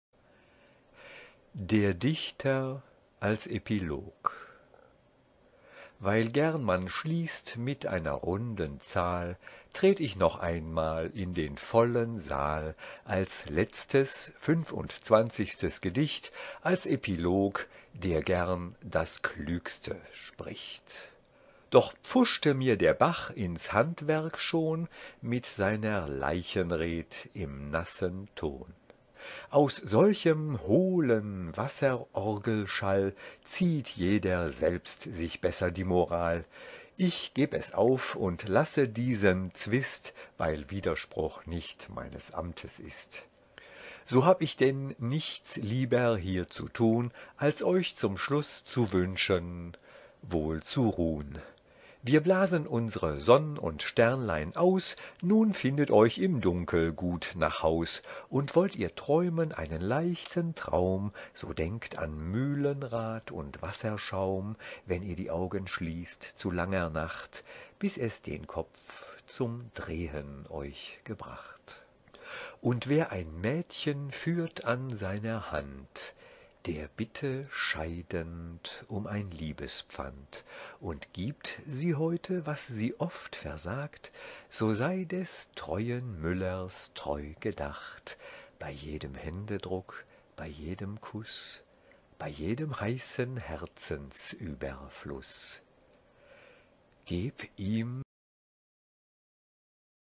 24 Epilog - Wilhelm Müller
Der Dichter, als Epilog________________ The Poet, in Epilogue

Weil gern man schließt mir einer runden Zahl, ______Because one loves to finish on a good round number
Tret' ich noch einmal in den vollen Saal, ____________I'm making yet another appearance in this filled hall,
Als letztes, fünf und zwanzigstes Gedicht, __________As the last, the twenty-fifth poem,
Als Epilog, der gern das Klügste spricht. ____________As the epilogue, which makes the cleverest comment.
Doch pfuschte mir der Bach in's Handwerk schon ___But the brook has already spoilt the show
Mit seiner Leichenred' im nassen Ton. _____________With its graveside speech in dampened tones.
Aus solchem hohlen Wasserorgelschall ____________From such a hollow water-organ sound
Zieht jeder selbst sich besser die Moral; ____________Each does well to draw his own conclusions;
Ich geb' es auf und lasse diesen Zwist, _____________I give up and leave you with the puzzle,
Weil Widerspruch nicht meines Amtes ist. __________Since contradiction is not within my compass.
So hab' ich denn nichts lieber hier zu tun, ___________Thus I have no nicer task than this:
Als euch zum Schluß zu wünschen, wohl zu ruhn. ___To wish you a very pleasant rest.
Wir blasen unsre Sonn' und Sternlein aus – _________We are going to blow out our sun and stars –
Nun findet auch im Dunkel gut nach Haus, __________Now may you find your way home safely,
Und wollt ihr träumen einen leichten Traum, ________And should you wish to dream a dainty dream,
So denkt an Mühlenrad und Wasserschaum, ________Then think of mill-wheels and foaming water
Wenn ihr die Augen schließt zu langer Nacht, _______When you close your eyes and the long night begins,
Bis es den Kopf zum Drehen euch gebracht, ________Until your head begins to spin with thinking of it;
Und wer ein Mädchen führt an seiner Hand, ________And he who holds a maiden by the hand,
Der bitte scheidend um ein Liebespfand, ___________Let him ask as he leaves for a love-token,
Und gibt sie heute, was sie oft versagt, _____________And if she gives today what she has often refused,
So sei des treuen Müllers treu gedacht _____________So think of the miller's constancy
Bei jedem Händedruck, bei jedem Kuß, _____________With every touch of your hands, with every kiss,
Bei jedem heißen Herzensüberfluß: ________________With every burning gesture from an overflowing heart:
Geb' ihm die Liebe für sein kurzes Leid _____________May love give him in return for his brief sorrow
In eurem Busen lange Seligkeit! ____________________Long-lasting bliss within your hearts!